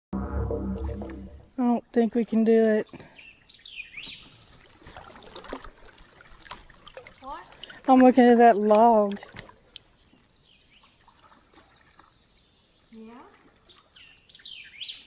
I always give up too easy 😏
From where I was sitting, it looked like we could not cross over the "log" (fallen tree). I'm glad I'm not the leader.